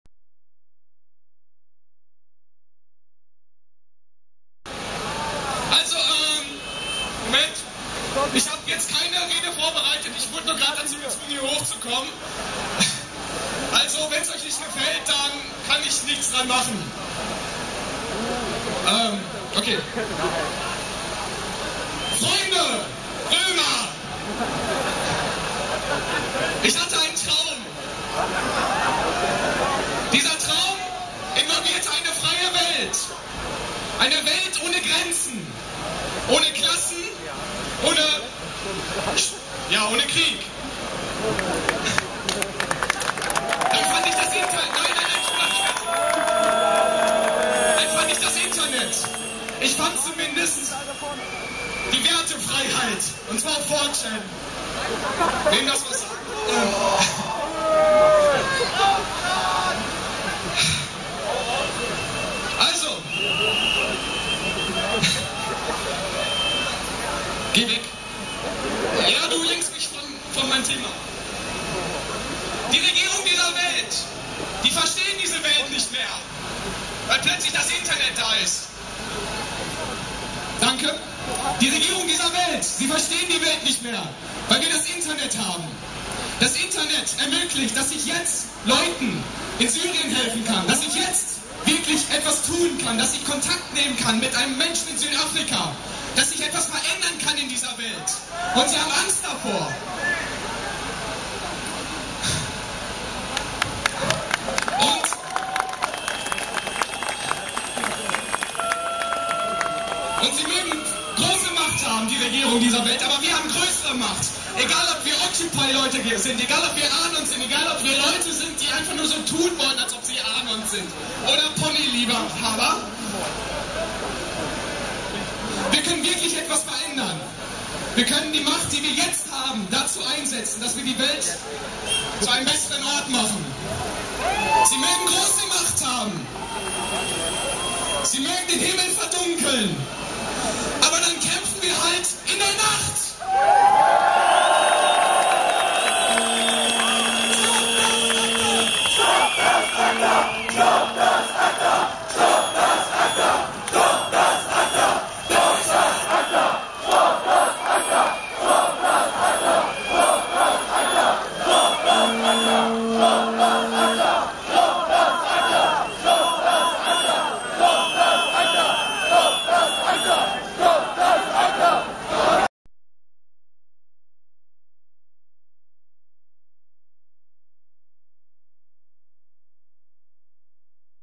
STOP ACTA Level 2 Demo in Düsseldorf: Unbekannter Redebeitrag [25.02.2012]
Ein weiterer Redebeitrag der *STOP ACTA (Level2) Demo* in Düsseldorf. Leider ist mir der Name des Redners nicht bekannt. Danach setzte sich die Demonstration wieder in Richtung Landtag in bewegung.